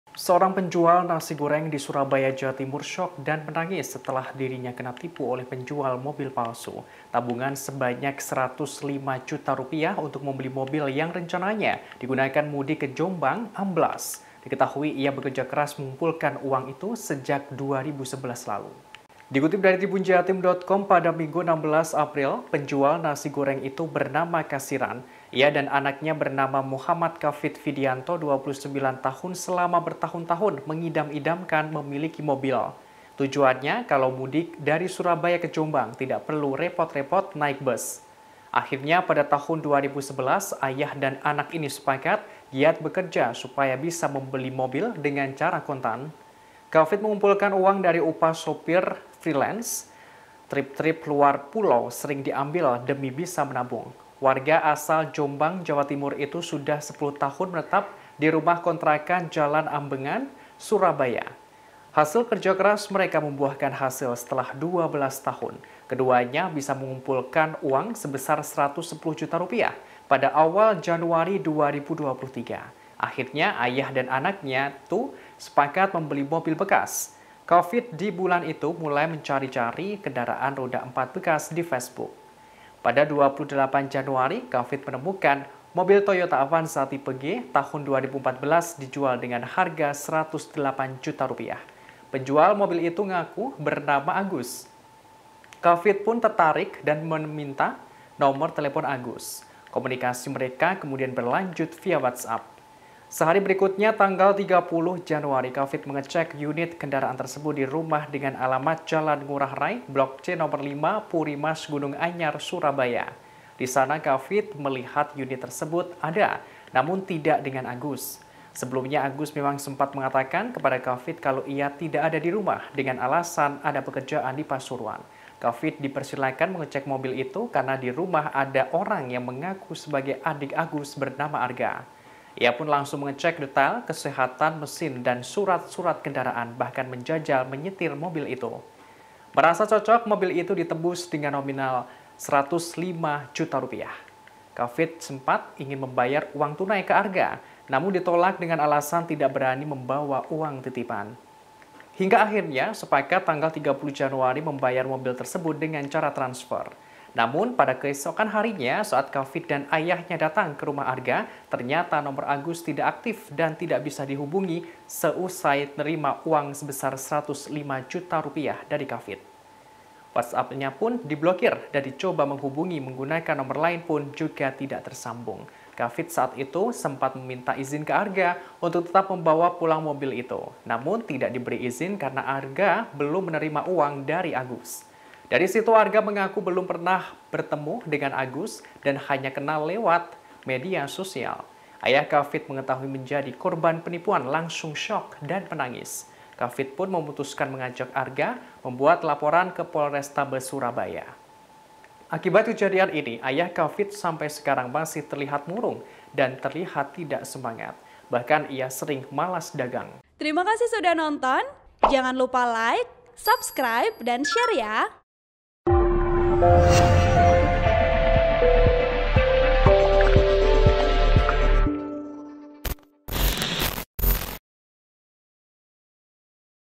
Ingin Beli Mobil, Penjual Nasgor di Surabaya Syok & Nangis Kena Tipu, Tabungan Rp 105 Juta Amblas
TRIBUN-VIDEO.COM - Seorang penjual nasi goreng di Surabaya syok dan menangis setelah dirinya kena tipu oleh penjual mobil palsu.

Tabungan Rp 105 juta untuk membeli mobil yang rencananya digunakan mudik ke Jombang amblas.

Diketahui, ia bekerja keras mengumpulkan uang sejak 2011.

Dikutip dari TribunJatim.com pada Minggu (16/4/2023), penjual nasi goreng itu bernama Kasiran.

Ia dan anaknya Muchammad Khafid Fidianto (29) selama bertahun-tahun mengindam-idamkan memiliki mobil.

Tujuannya, kalau mudik dari Surabaya ke Jombang tidak perlu repot-repot naik bus.

Akhirnya pada 2011, ayah dan anak ini sepakat giat bekerja supaya bisa membeli mobil dengan cara kontan.

Khafid mengumpulkan uang dari upah menjadi sopir freelance.

Trip-trip luar pulau sering diambil demi bisa menabung.

Warga asal Jombang, Jawa Timur itu sudah 10 tahun menetap di rumah kontrakan Jalan Ambengan Surabaya.

Hasil kerja keras mereka membuahkan hasil setelah 12 tahun.

Keduanya bisa mengumpulkan uang sebesar Rp110 juta pada awal Januari 2023 lalu.

Akhirnya, ayah dan anaknya sepakat membeli mobil bekas.

Khafid di bulan itu mulai cari-cari kendaraan roda empat bekas di Facebook.

Pada 28 Januari 2023, Khafid menemukan mobil Toyota Avanza tipe G tahun 2014 dijual dengan harga Rp 108 juta.

Penjual mobil itu ngaku bernama Agus.

Khafid pun tertarik dan meminta nomor telepon Agus.

Komunikasi mereka kemudian berlanjut via WhatsApp.

Sehari berikutnya tanggal 30 Januari, Khafid mengecek unit kendaraan tersebut di rumah dengan alamat Jalan Ngurah Rai Blok C No 5, Puri Mas Gunung Anyar, Surabaya.

Di sana Khafid melihat unit tersebut ada, namun tidak dengan Agus.

Sebelumnya, Agus memang sempat mengatakan kepada Khafid kalau ia tidak ada di rumah dengan alasan ada pekerjaan di Pasuruan.

Khafid dipersilakan mengecek mobil itu karena di rumah ada orang yang mengaku sebagai adik Agus bernama Arga.

Ia pun langsung mengecek detail kesehatan mesin dan surat-surat kendaraan bahkan menjajal menyetir mobil itu.

Merasa cocok, mobil itu ditebus dengan nominal Rp 105 juta.

Khafid sempat ingin membayar uang tunai ke Arga, namun ditolak dengan alasan tidak berani membawa uang titipan.

Hingga akhirnya sepakat tanggal 30 Januari membayar mobil tersebut dengan cara transfer.

Namun, pada keesokan harinya saat Khafid dan ayahnya datang ke rumah Arga, ternyata nomor Agus tidak aktif dan tidak bisa dihubungi seusai terima uang sebesar Rp 105 juta dari Khafid.

"Aku sempat mau bayar tunai ke Arga. Tapi sama Arga ditolak, alasannya gak berani karena hanya dipasrahi Agus titip unit. Aku akhirnya sepakat tanggal 30 Januari membayar mobil tersebut dengan cara transfer," ujar Khafid.

WhatsAppnya diblokin dan dicoba menghubungi menggunakan nomor lain pun juga tidak tersambung.

Khafid saat itu sempat meminta izin ke Arga untuk tetap membawa pulang mobil tersebut.

Namun, tidak diberi izin karena Arga belum menerima uang dari Agus.

"Di situ Arga baru ngaku belum pernah ketemu Agus.

Ayah Khafid mengetahui menjadi korban penipuan langsung syok dan menangis.

Khafid pun memutuskan mengajak Arga membuat laporan ke Polrestabes Surabaya.

Akibat kejadian ini, ayah Khafid sampai sekarang masih terlihat murung dan terlihat tidak semangat.

Bahkan, ia sering malas dagang.

(Tribun-Video.com/ TribunJatim.com)